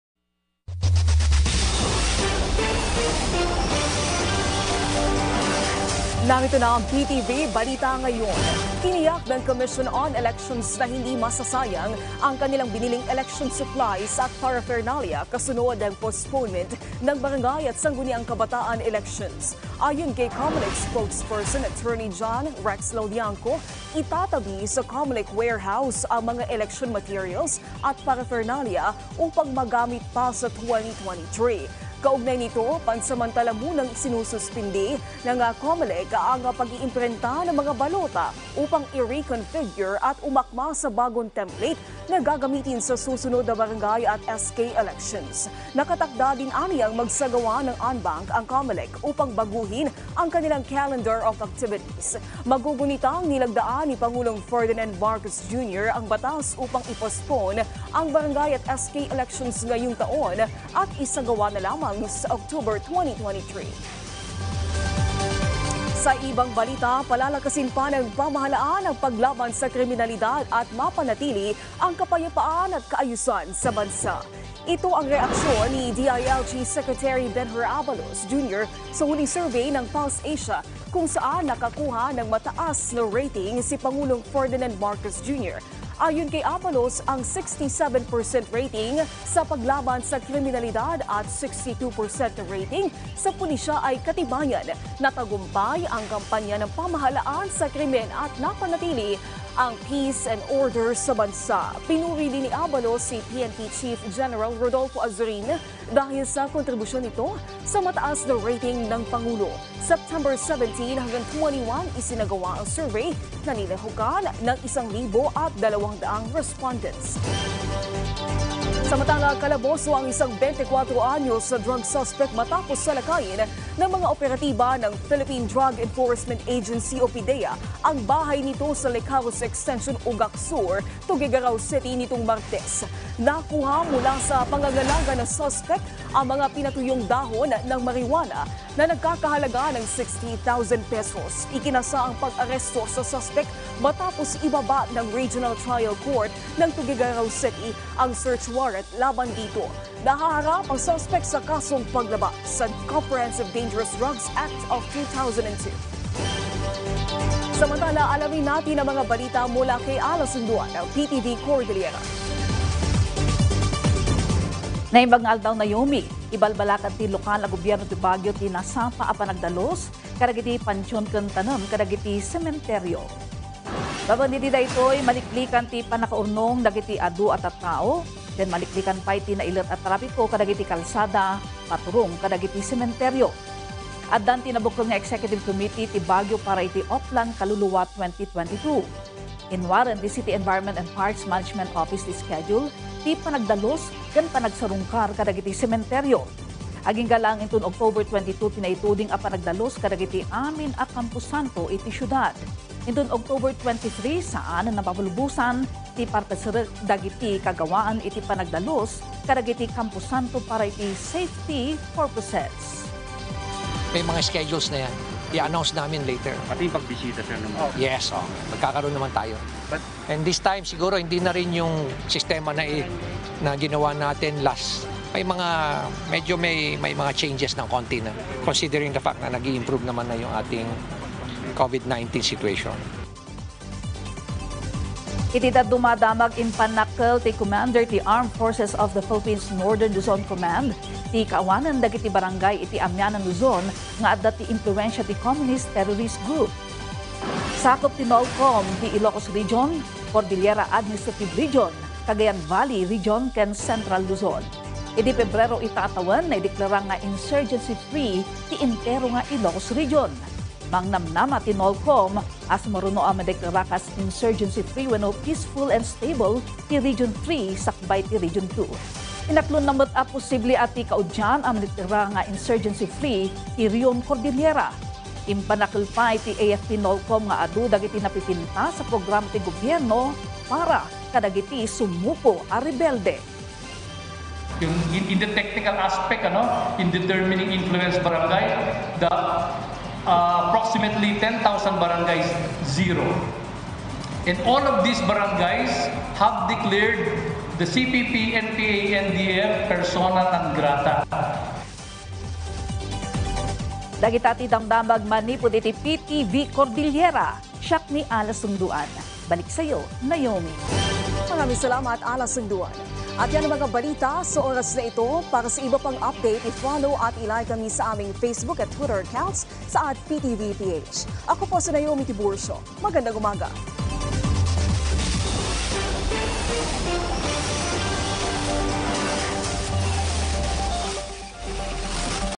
Comelec, tiniyak na hindi masasayang ang kanilang election materials kasunod ng postponement ng BSKE
PTVBalitaNgayon | Comelec, tiniyak na hindi masasayang ang kanilang election materials kasunod ng postponement ng BSKE;

Pamahalaan, paiigtingin pa ang paglaban sa kriminalidad;

Drug suspect, kalaboso sa Tuguegarao City